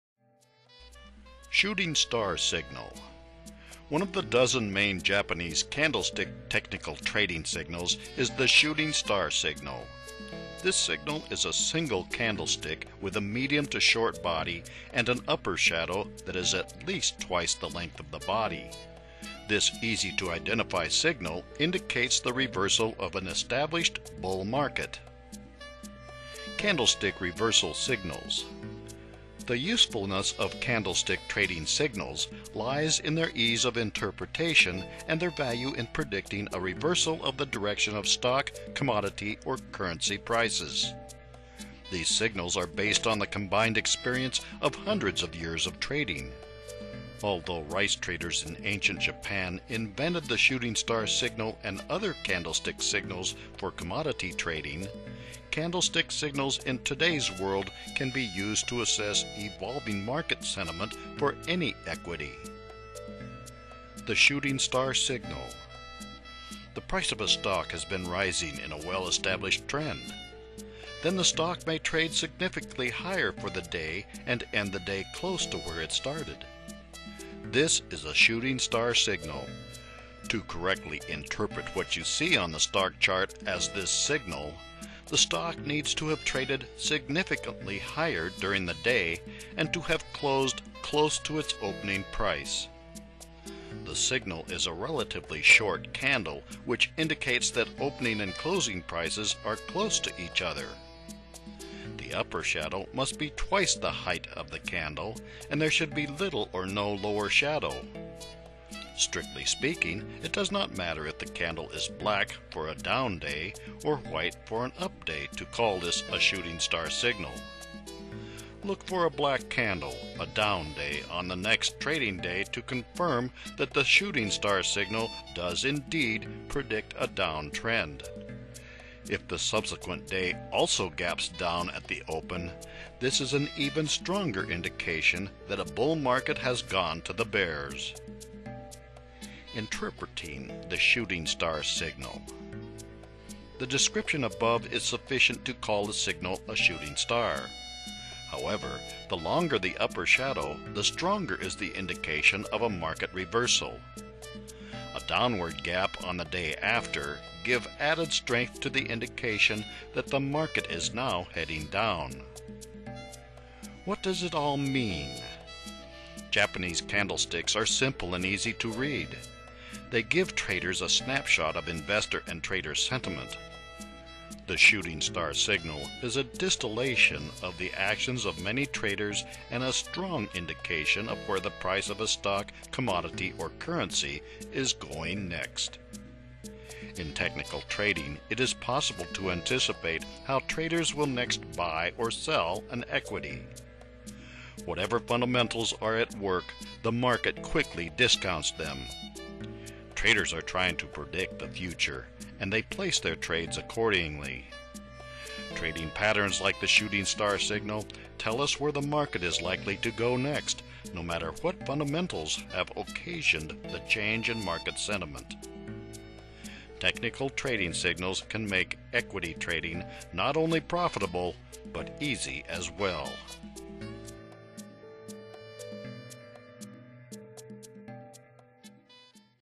Shooting Star Signal
One of the dozen main Japanese candlestick technical trading signals is the Shooting Star Signal. This signal is a single candlestick with a medium to short body and an upper shadow that is at least twice the length of the body. This easy to identify signal indicates the reversal of an established bull market.

Candlestick Reversal Signals

The usefulness of candlestick trading signals lies in their ease of interpretation and their value in predicting a reversal of the direction of stock, commodity, or currency prices. These signals are based on the combined experience of hundreds of years of trading. Although rice traders in ancient Japan invented the Shooting Star Signal and other candlestick signals for commodity trading, candlestick signals in today's world can be used to assess evolving market sentiment for any equity.

The Shooting Star Signal

The price of a stock has been rising in a well established trend. Then the stock may trade significantly higher and end the day close to where it started. This is a Shooting Star Signal. To correctly interpret what you see on the stock chart as this signal the stock needs to have traded significantly higher during the day and to have closed close to its opening price. The signal is a relatively short candle which indicates that opening and closing prices are close to each other. The upper shadow must be twice the height of the candle and there should be little or no lower shadow. Strictly speaking it does not matter if the candle is black for a down day or white for an up day to call this a Shooting Star Signal. Look for a black candle, a down day, on the next trading day to confirm that the Shooting Star Signal does indeed predict a down trend. If the subsequent day also gaps down at the open this is an even stronger indication that a bull market has gone to the bears.

Interpreting the Shooting Star Signal

The description above is sufficient to call the signal a shooting star. However, the longer the upper shadow the stronger is the indication of a market reversal. A downward gap on the day after give added strength to the indication that the market is now heading down.

What Does It All Mean?

Japanese candlesticks are simple and easy to read. They give traders a snapshot of investor and trader sentiment. The Shooting Star Signal is a distillation of the actions of many traders and a strong indication of where the price of a stock, commodity, or currency is going next. In technical trading it is possible to anticipate how traders will next buy or sell an equity. Whatever fundamentals are at work the market quickly discounts them. Traders are trying to predict the future and they place their trades accordingly. Trading patterns like the Shooting Star Signal tell us where the market is likely to go next, no matter what fundamentals have occasioned the change in market sentiment. Technical trading signals can make equity trading not only profitable but easy as well.